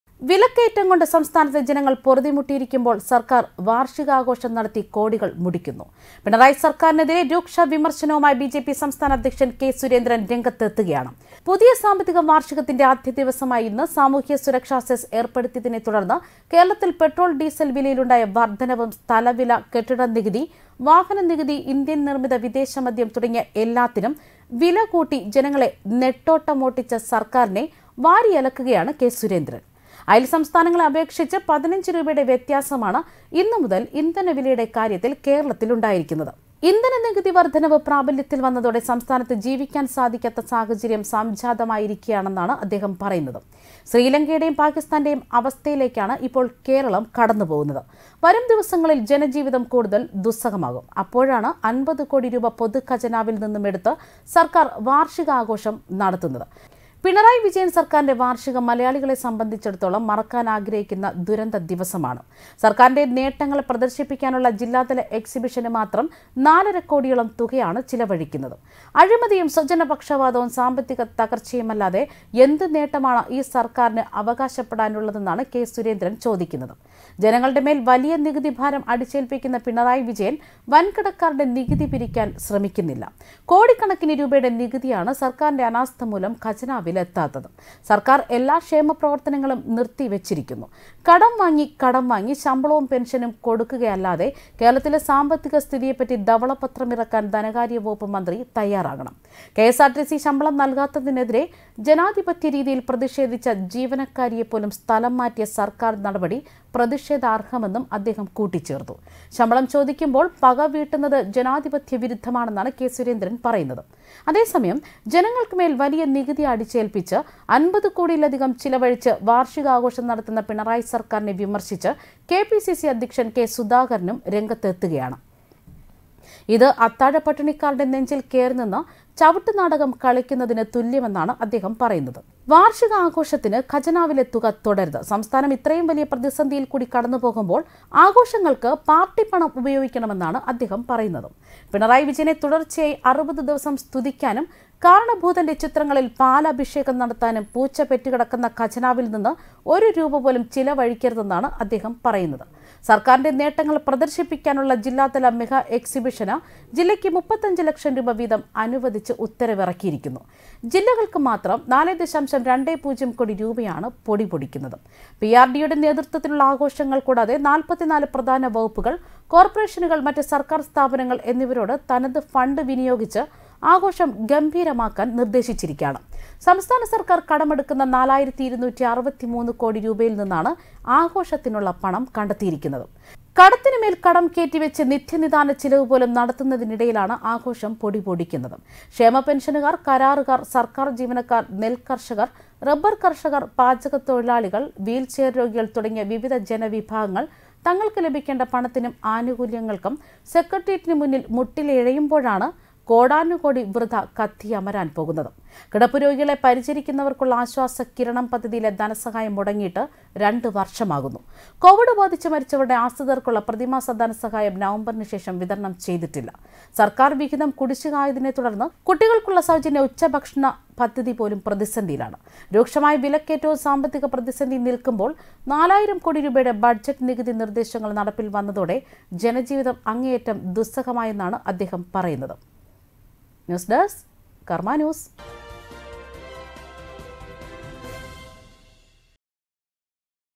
ജനങ്ങളെ മുടിച്ചു സർക്കാർ വാർഷികാഘോഷത്തിന് കോടികൾ മുടിക്കുന്നു
വിലക്കയറ്റം കൊണ്ട് സംസ്ഥാനത്തെ ജനങ്ങൾ പൊറുതിമുട്ടിച്ചു പിണറായി സർക്കാർ വാർഷികാഘോഷം നടത്താൻ ഇറക്കുന്നത് കോടികൾ